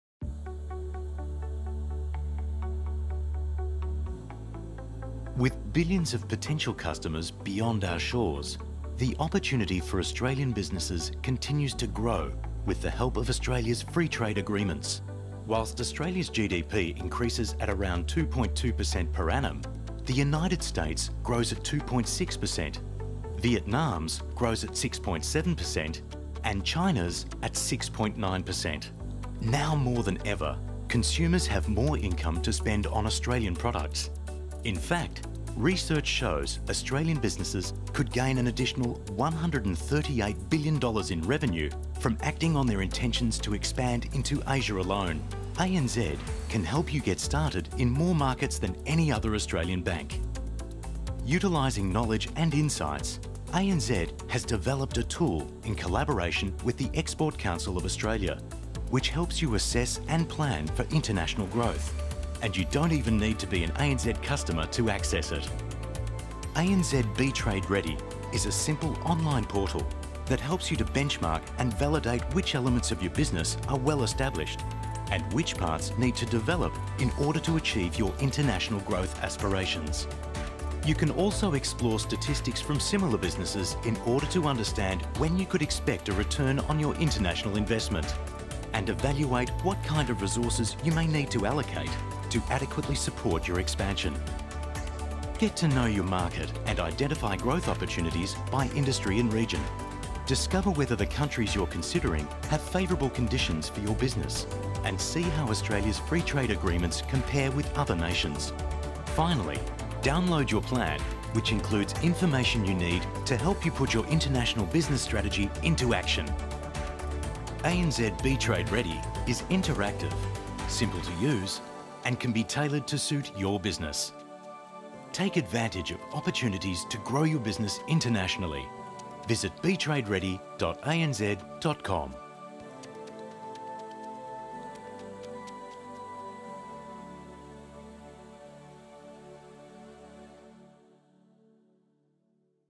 Assess and plan your international expansion with ANZ Be Trade Ready
Looking to grow your business overseas? We’ve launched a new tool to help businesses plan for international expansion called ANZ Be Trade Ready.